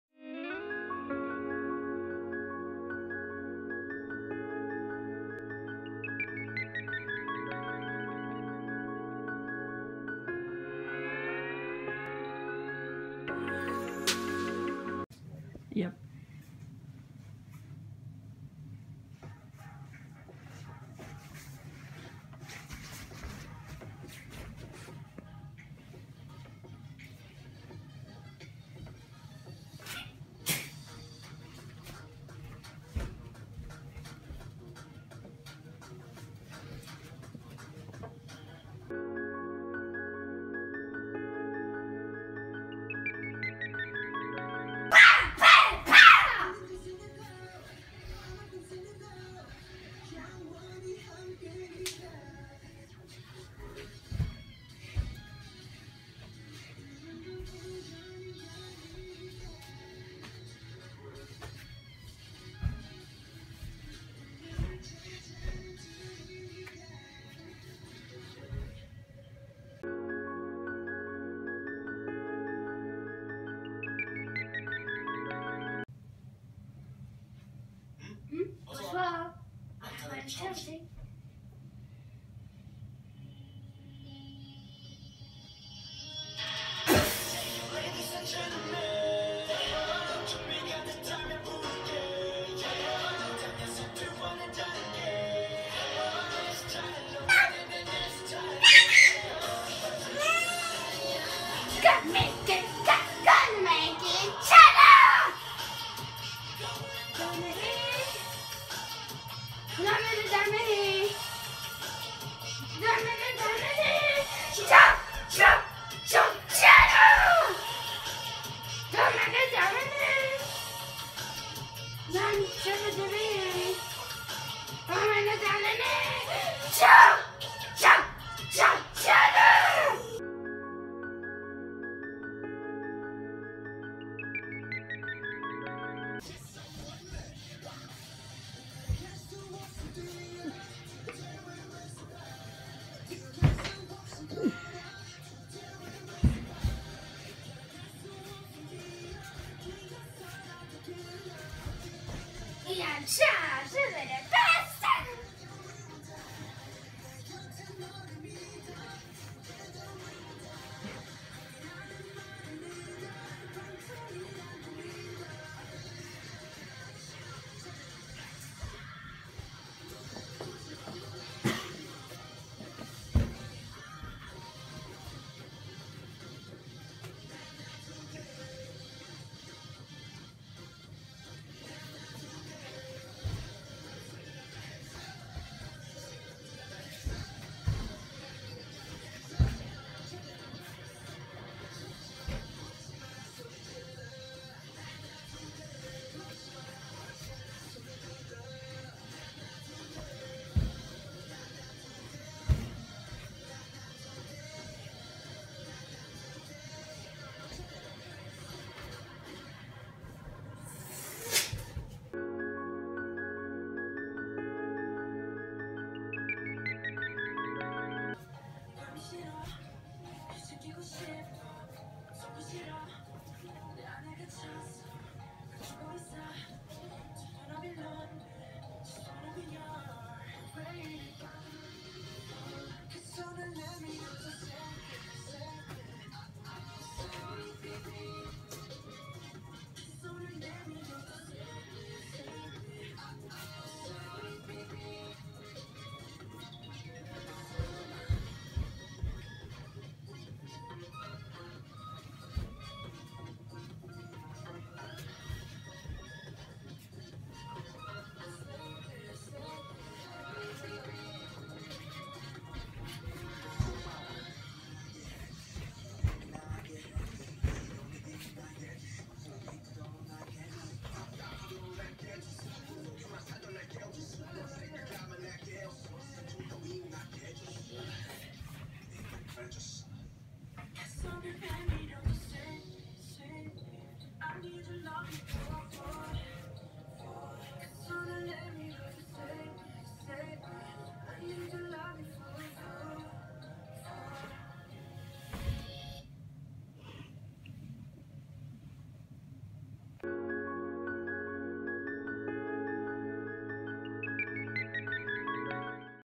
BTS Baby Army Trying to imitate BTS!!!
in this Video, BTS Baby Army Julien tries hard to imitate BTS choreographies. Remember this is a little boy with disabilities. I hope you enjoy!